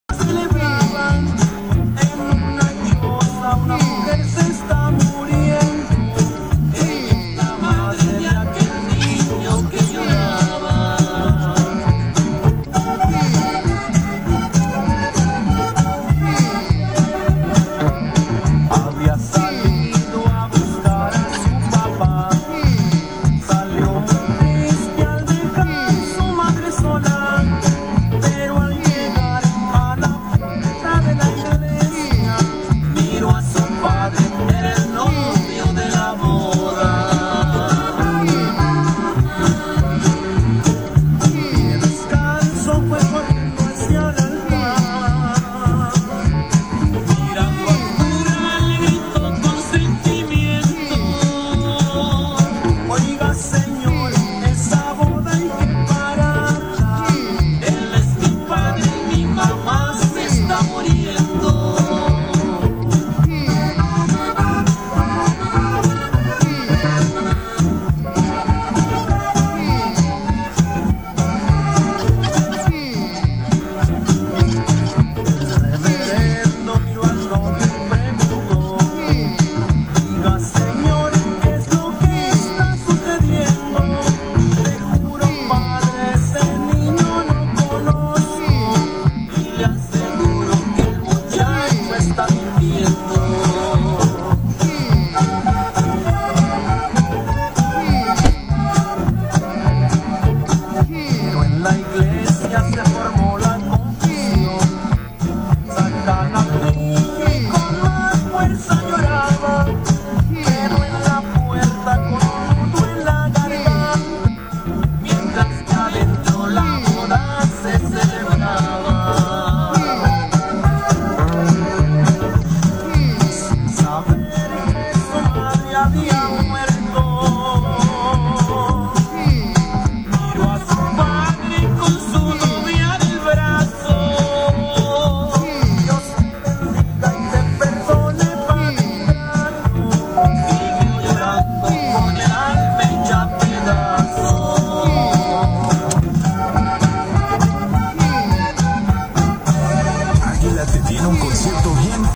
MAX THINKS WHILE I PLAY UNKNOWN BAILANDO EN TEJAS SONG #19
This is also a Radio Aguila/Bailando En Tejas music video.